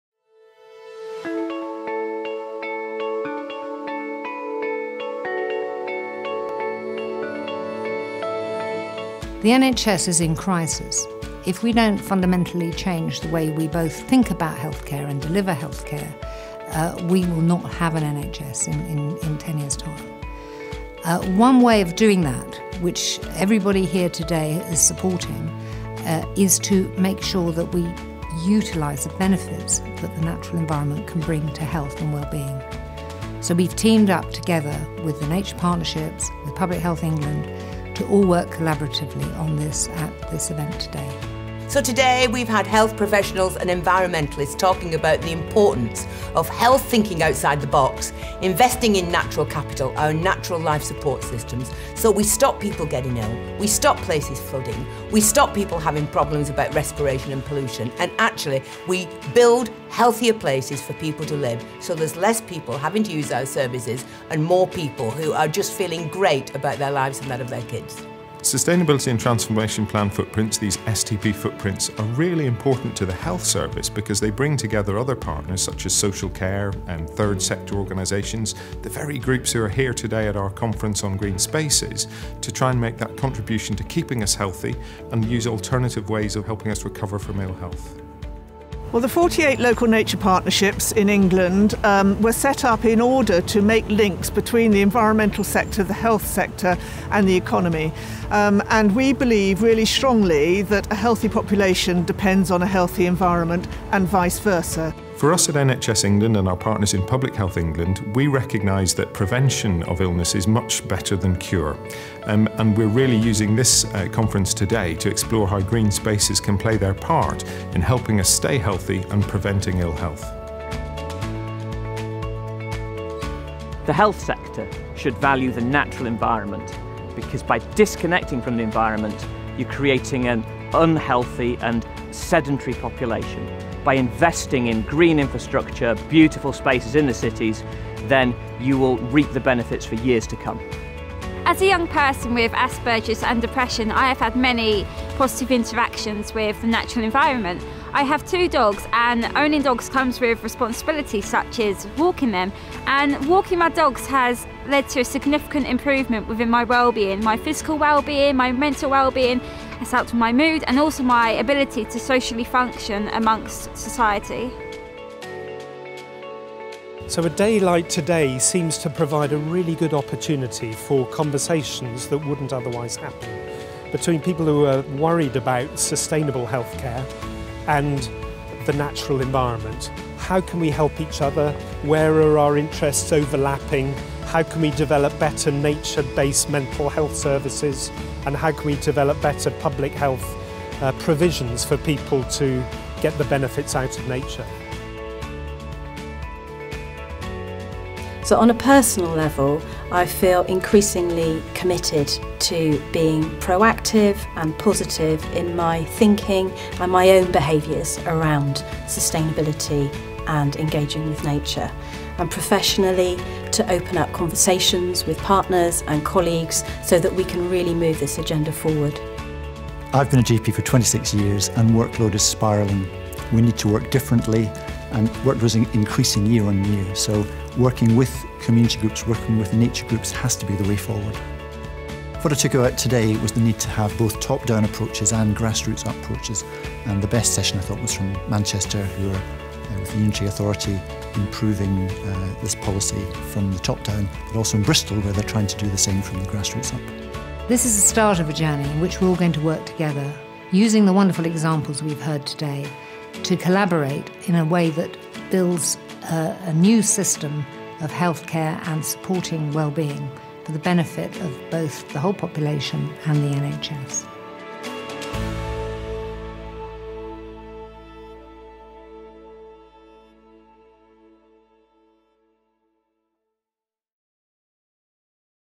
Delivering the Health and Wellbeing Benefits of the Natural Environment
Highlights from a conference run by the South West Local Nature Partnerships, NHS England and Public Health England, exploring the health benefits of the natural environment.